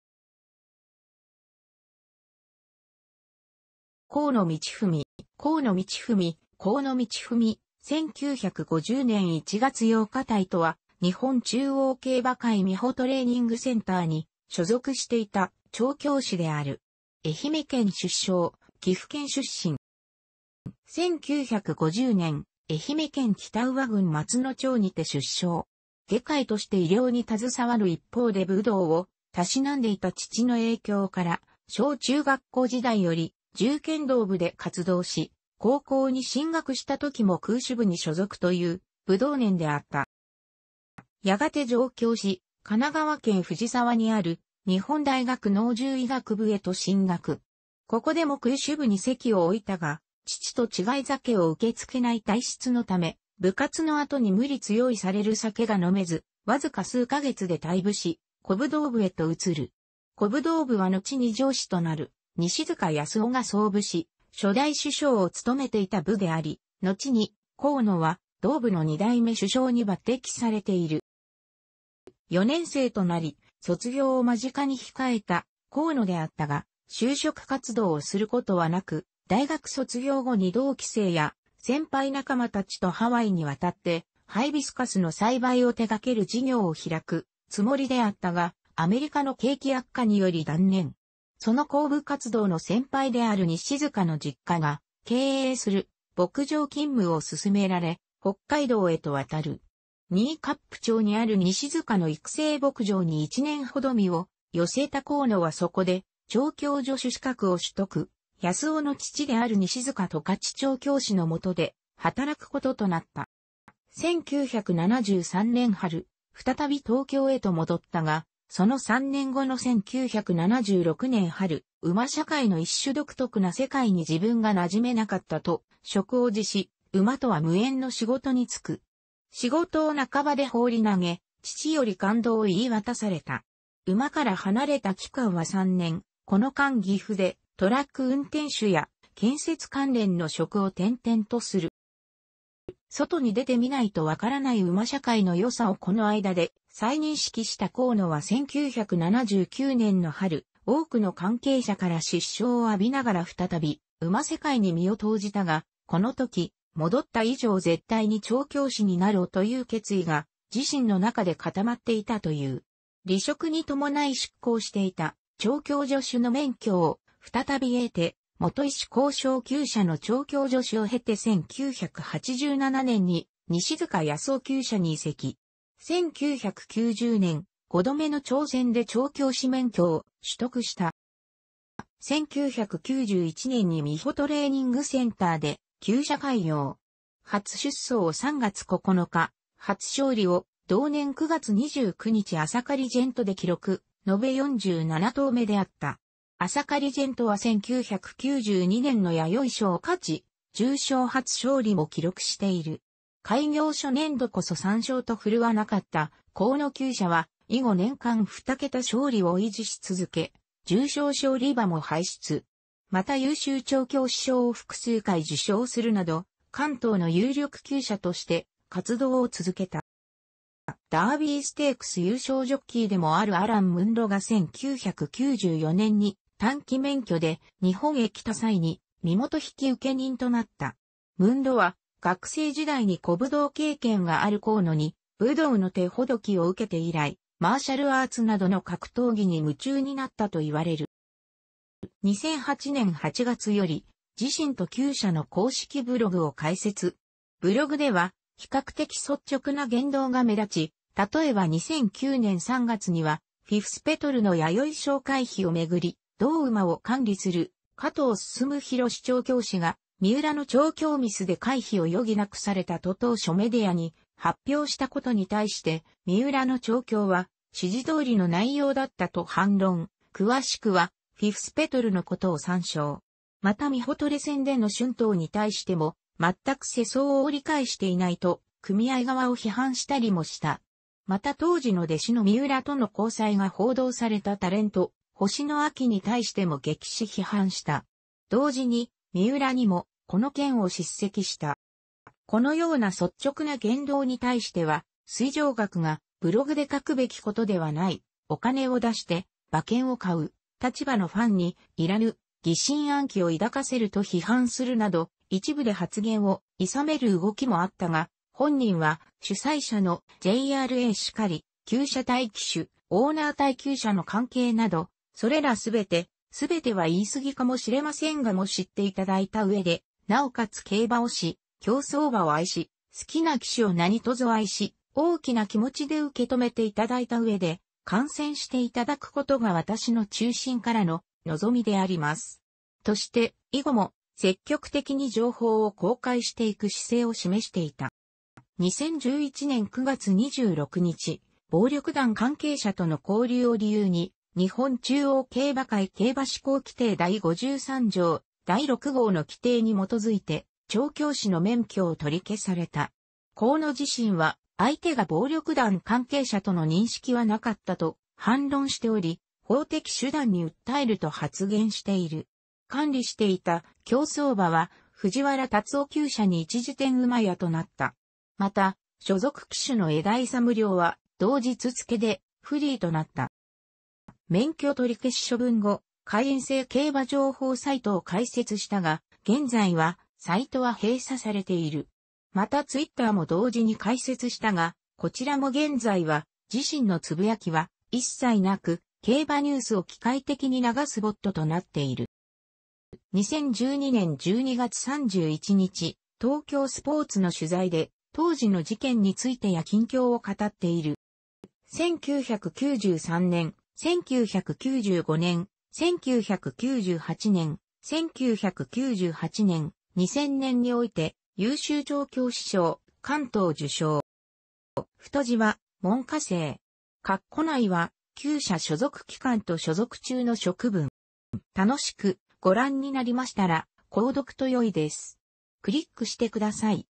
河野通文
河野 通文（こうの みちふみ、1950年1月8日 - ）とは日本中央競馬会美浦トレーニングセンターに所属していた調教師である。愛媛県出生、岐阜県出身。

1950年、愛媛県北宇和郡松野町にて出生。外科医として医療に携わる一方で武道を嗜んでいた父の影響から、小中学校時代より柔剣道部で活動し、高校に進学したときも空手部に所属という武道少年であった。

やがて上京し、神奈川県藤沢にある日本大学農獣医学部へと進学。ここでも空手部に籍を置いたが、父と違い酒を受け付けない体質のため、部活のあとに無理強いされる酒が飲めず、わずか数か月で退部し古武道部へと移る。古武道部はのちに上司となる西塚安夫が創部し初代主将を務めていた部であり、のちに河野は同部の2代目主将に抜擢されている。

4年生となり卒業を間近に控えた河野であったが就職活動をすることはなく、大学卒業後に同期生や先輩仲間達とハワイに渡ってハイビスカスの栽培を手がける事業を拓くつもりであったが、アメリカの景気悪化により断念。その後部活動の先輩である西塚の実家が経営する牧場勤務を勧められ、北海道へと渡る。新冠町にある西塚の育成牧場に1年ほど身を寄せた河野はそこで調教助手の資格を取得、安夫の父である西塚十勝調教師のもとで働くこととなった。

1973年春、ふたたび東京へと戻ったが、その3年後の1976年春、「馬社会の一種独特な世界に自分が馴染めなかった」と職を辞し、馬とは無縁の仕事に就く。仕事を半ばで放り投げ、父より勘当を言い渡された。馬から離れた期間は3年、この間岐阜でトラック運転手や建設関連の職を転々とする。

「外に出てみないとわからない馬社会のよさ」をこの間で再認識した河野は1979年の春、多くの関係者から失笑を浴びながらふたたび馬世界に身を投じたが、このとき「戻った以上絶対に調教師になろう」という決意が自身の中で固まっていたという。離職に伴い失効していた調教助手の免許をふたたび得て、元石孝昭厩舎の調教助手を経て1987年に西塚安夫厩舎に移籍。1990年、5度目の挑戦で調教師免許を取得した。

1991年に美浦トレーニングセンターで厩舎開業。初出走を3月9日、初勝利を同年9月29日アサカリジェントで記録、のべ47頭目であった。アサカリジェントは1992年の弥生賞を勝ち、重賞初勝利も記録している。開業初年度こそ3勝と奮わなかった河野厩舎は以後年間2桁勝利を維持し続け、重賞勝利馬も輩出。また優秀調教師賞を複数回受賞するなど関東の有力厩舎として活動を続けた。

ダービーステークス優勝ジョッキーでもあるアラン・ムンロ (Alan Munro) が1994年に短期免許で日本へ来た際に身元引受人となった。ムンロは、学生時代に古武道経験がある河野に武道の手ほどきを受けて以来、マーシャルアーツなどの格闘技に夢中になったといわれる。

2008年8月より、自身と厩舎の公式ブログを開設。ブログでは比較的率直な言動が目立ち、例えば2009年3月にはフィフスペトルの弥生賞回避を巡り、同馬を管理する加藤征弘調教師が「三浦の調教ミスで回避を余儀なくされた」と当初メディアに発表したことに対して「三浦の調教は指示通りの内容だった」と反論（詳しくはフィフスペトルの記事を参照）。また美浦トレセンでの春闘に対しても「全く世相を理解していない」と組合側を批判したりもした。また当時の弟子の三浦との交際が報道されたタレント・ほしのあきに対しても激しく批判した。同時に三浦にもこの件を叱責した。

このような率直な言動に対しては、水上学が「ブログで書くべきことではない」「お金を出して馬券を買う立場のファンに、要らぬ疑心暗鬼を抱かせる」と批判するなど、一部で発言を諌める動きもあったが、本人は「主催者のJRAしかり、厩舎対騎手、オーナー対厩舎の関係など、それら全て（全ては言い過ぎかもしれませんが）も知って頂いた上で尚且つ競馬を愛し、競走馬を愛し、好きな騎手を何卒愛し、大きな気持ちで受け止めて頂いた上で、観戦して頂く事が私の衷心からの望みであります。」として、以後も積極的に情報を公開していく姿勢を示していた。

2011年9月26日、暴力団関係者との交流を理由に、日本中央競馬会競馬施行規程第53条第6号の規定に基づいて、調教師の免許を取り消された。河野自身は相手が暴力団関係者との認識はなかったと反論しており、法的手段に訴えると...